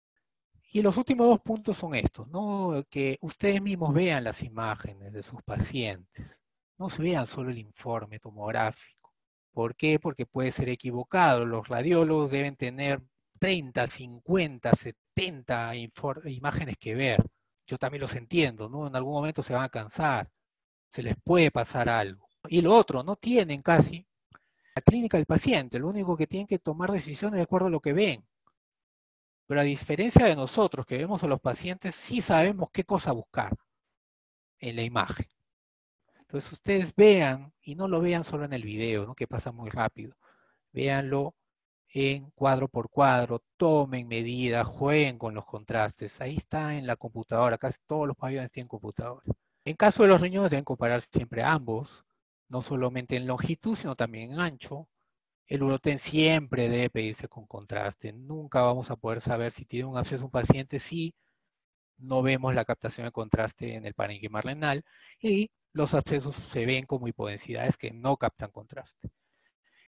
Análisis de Tomografía en sospecha de abscesos renales
Algunas recomendaciones para evaluar los casos de nuestros pacientes analizando nosotros mismos sus imágenes, haciendo énfasis en la tomografía buscando abscesos renales.